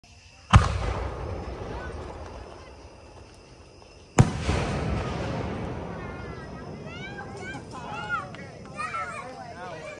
Silver palm over Knighton Blue
20lbs of silver palm made with scratch mix BP over 33lb of 1/2” Knighton blue.